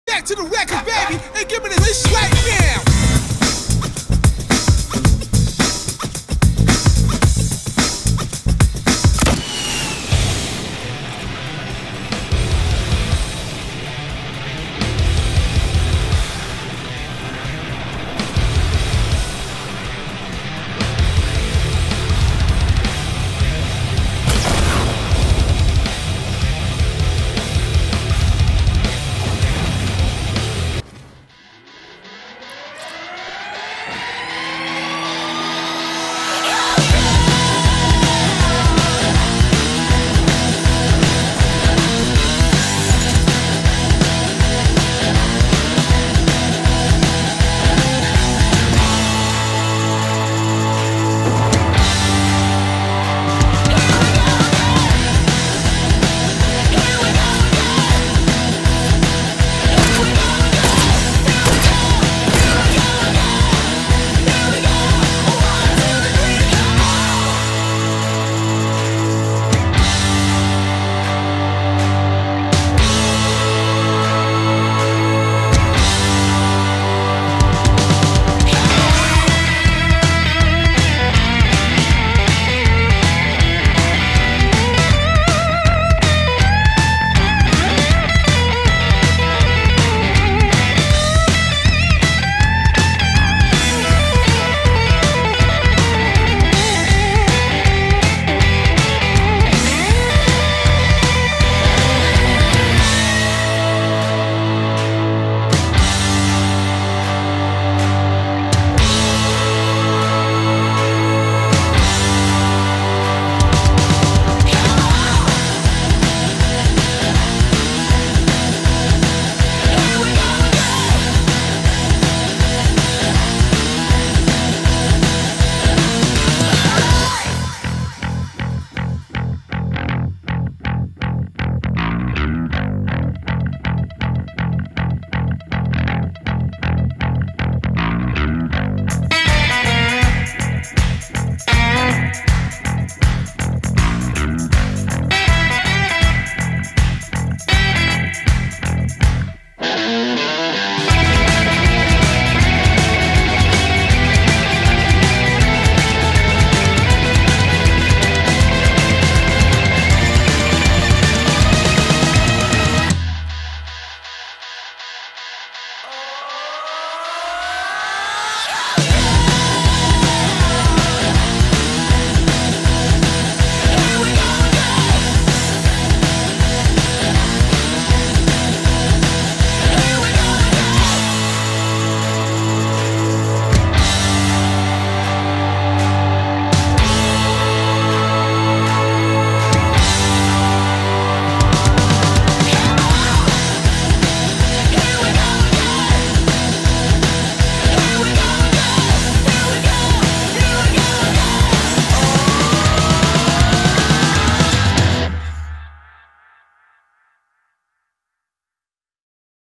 Epic rocket ride and no scope shot
Through the port a fort at the right time and got a sick no scope